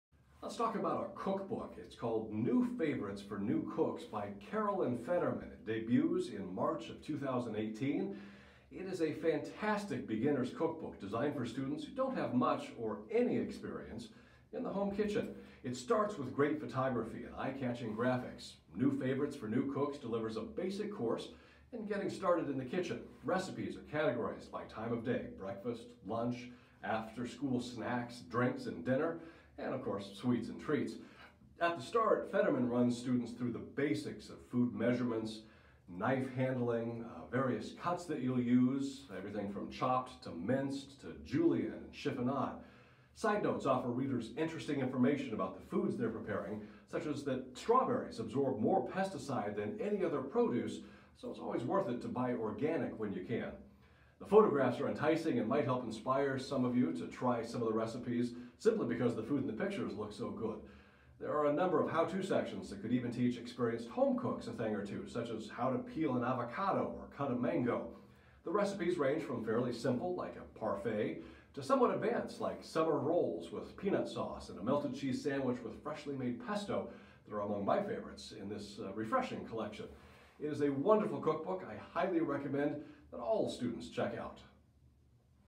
New Favorites for New Cooks 60 Book talk
Book review of the new non-fiction cookbook for students titled "New Favorites for New Cooks"